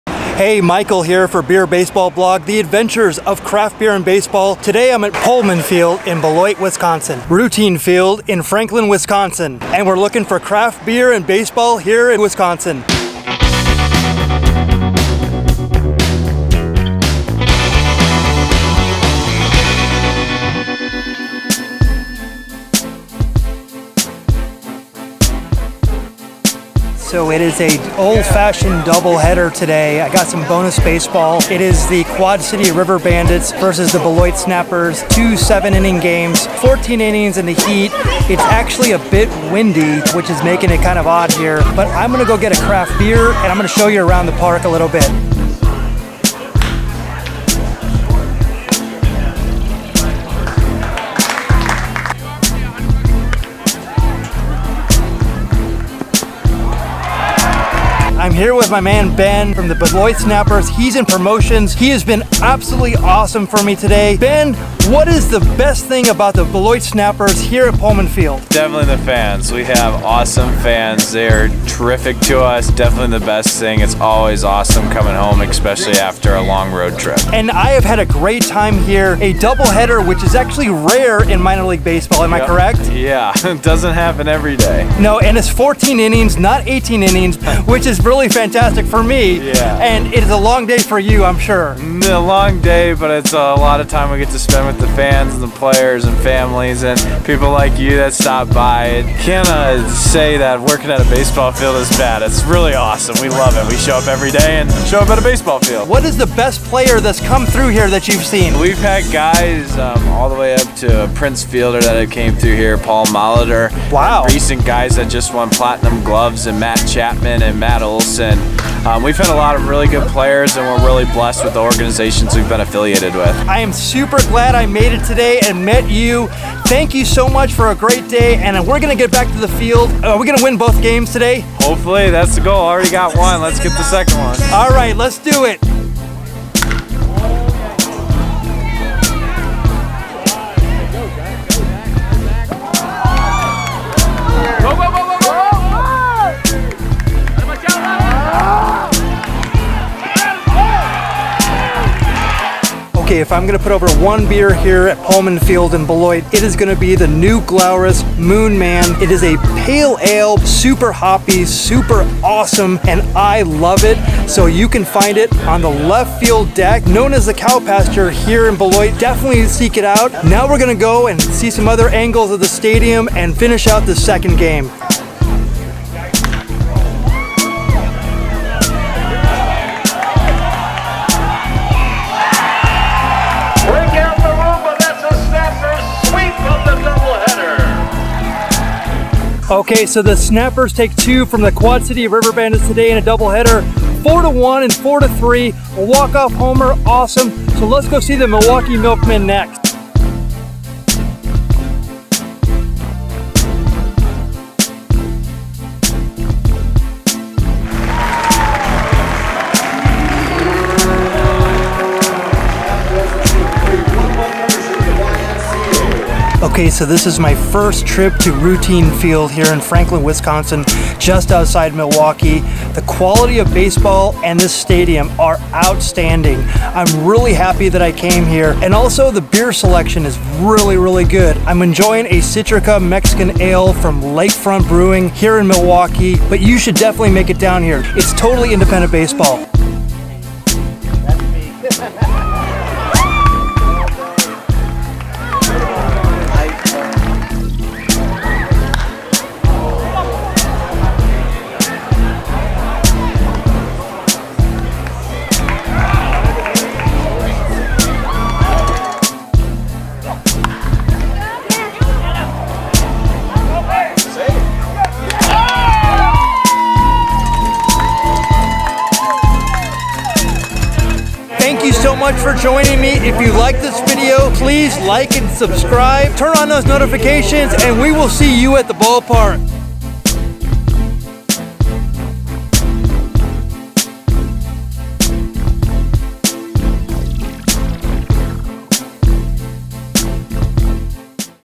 Hoppin' Around: Beloit Snappers & Milwaukee Milkmen – Wisconsin Beer & Baseball
Join Michael of Beer Baseball Blog as he visits the Pohlman Field to see the MiLB Beloit Snappers.

And he travels to  Routine Field to see the independent Milwaukee Milkmen. It is craft beer and baseball in Wisconsin.